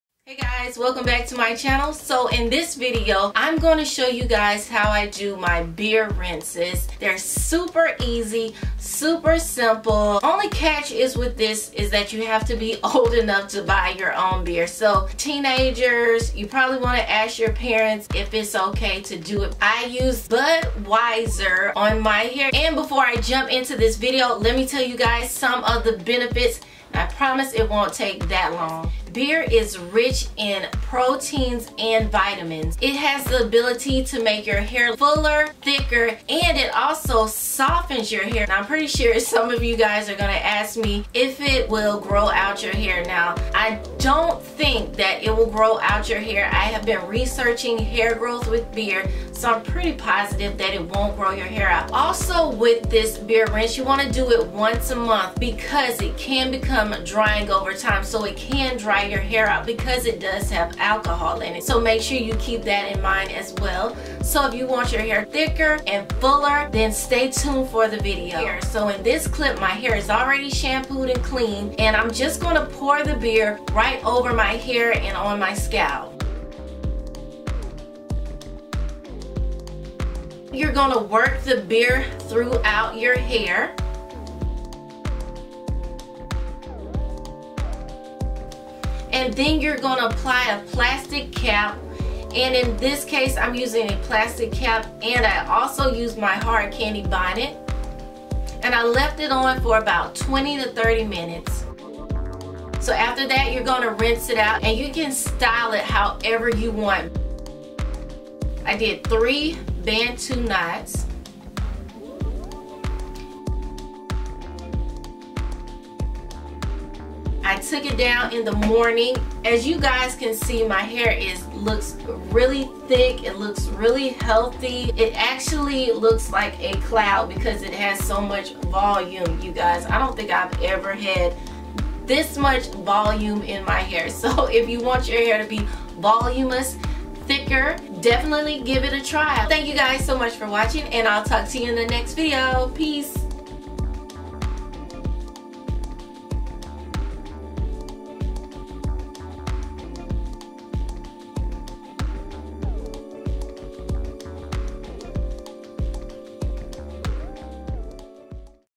BEER Hair Mask
Beer is rich in proteins and vitamins. Beer makes hair fuller, thicker, and softer.  Apply this once a month, because it can become dryer over time.
Pour beer on shampooed hair, work through hair and scalp. Apply a plastic cap and leave on for 25 mins.  Rinse and style as usual.

Thank you to all the people who 👍🏾 this video I love you so much and thanks for the support 💪🏾 it encourages me to make more videos ☝